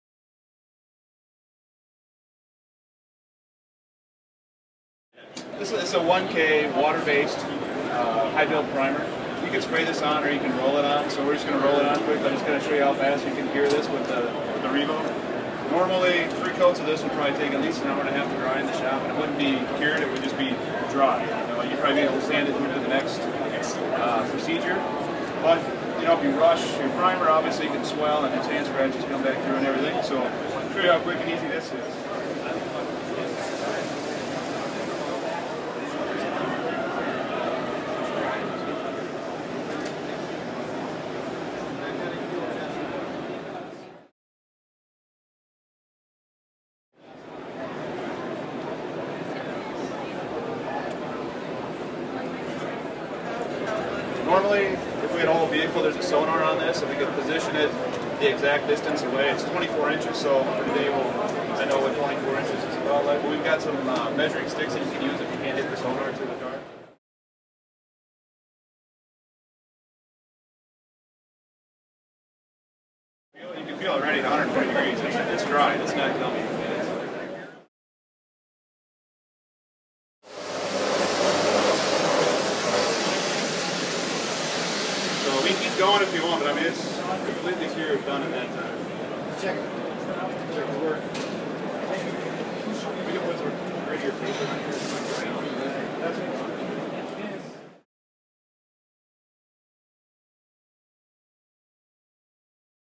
GFS REVO DEMO SEMA 2015 Accelerated Curing Systems
Global Finishing Solutions (GFS) demonstrated its REVO accelerated curing systems during SEMA. Introduced to the industry this past year, all four REVO products utilize short wave electric infrared technology.
Demonstration done by Brandon Lowder, VP of Auto Refinish Sales for GFS.